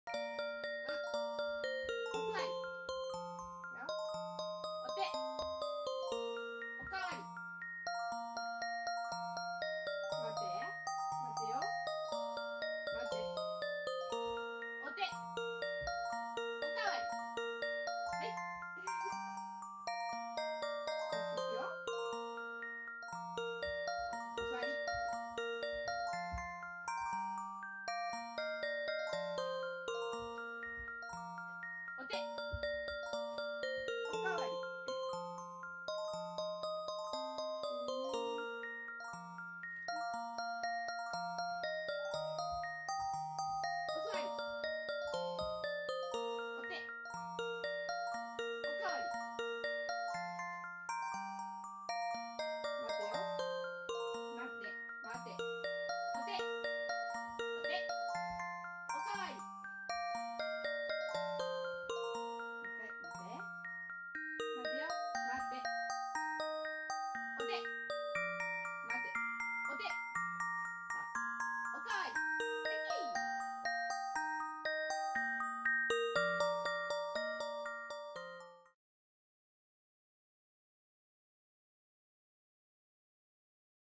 おかわりができたよ
バーニーズマウンテンドッグの

ブラウニー(♂)

生後5ヶ月と7日

おかわりができるようになりました